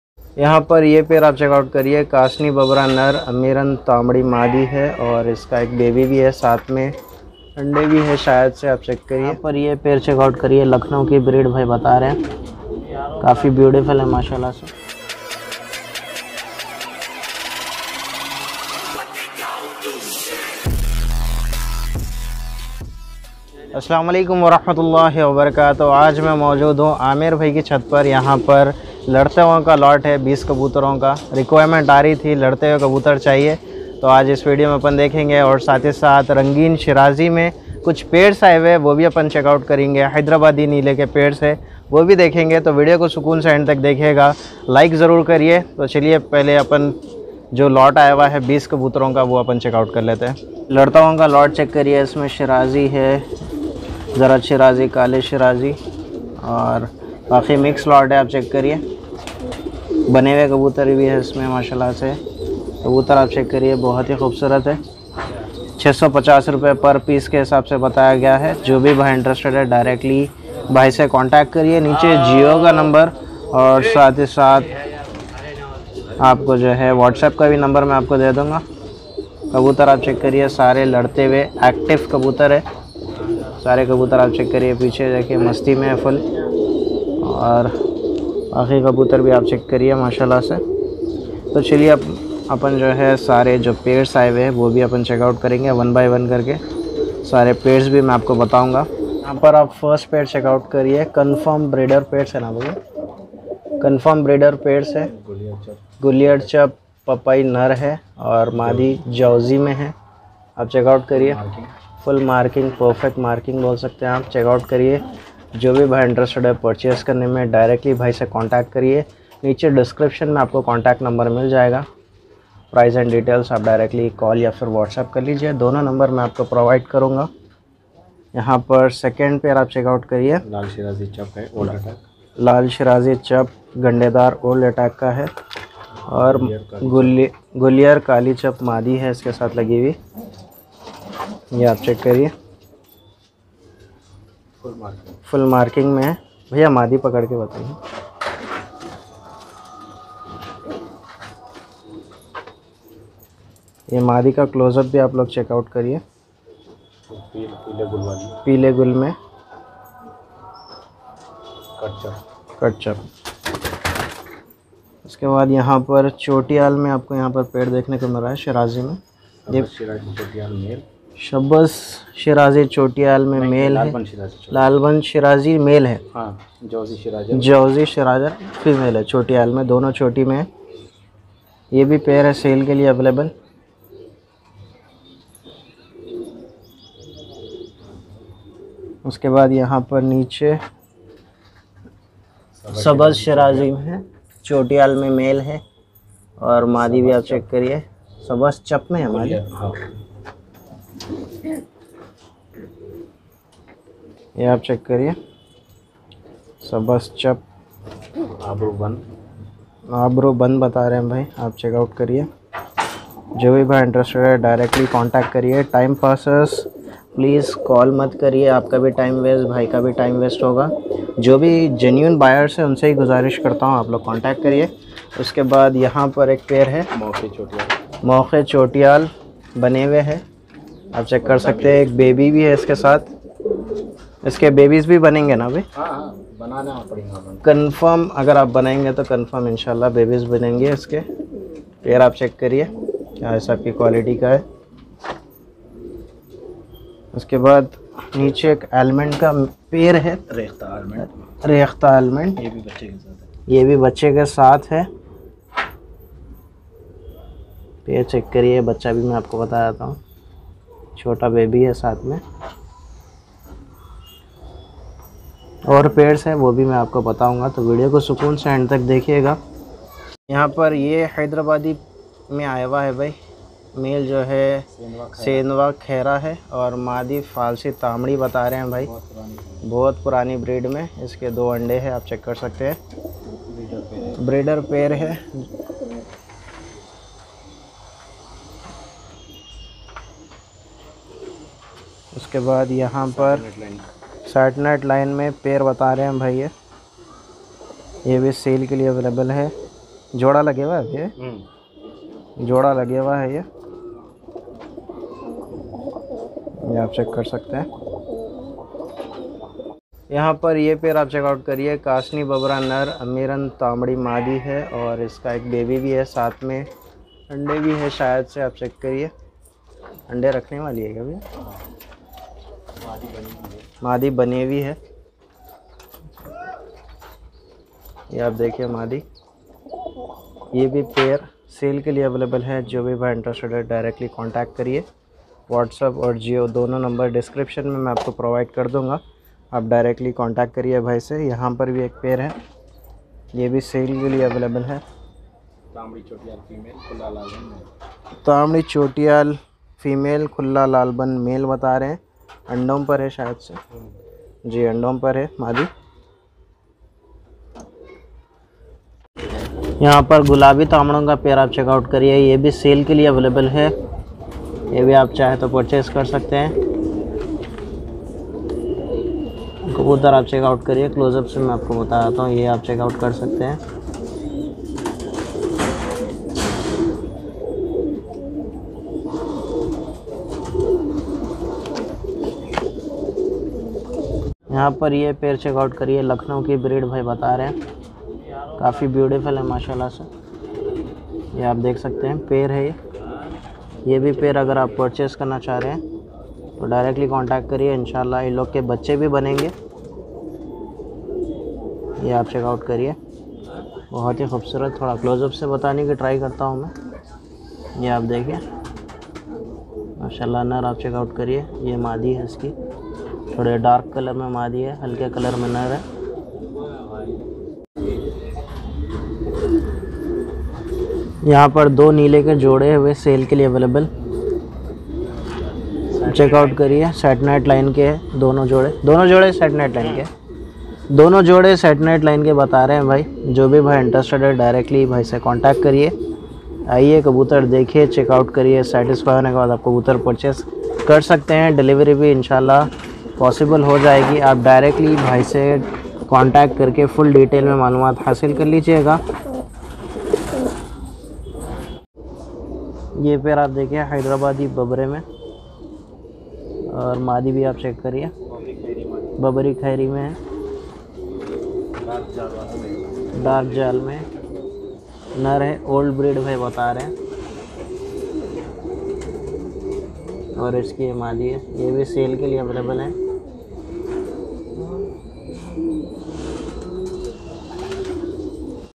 Rangeen Sherazi Breeding pairs For Sale🤩 || Larty Hue Kabootar For sale 🤑 in Hyd Deccan
Assalamualaikum , Aadaab.
Amair Bhai
Location : karwan , Tappachabutra.
MH pigeons ko Instagram pr Follow karen 👇.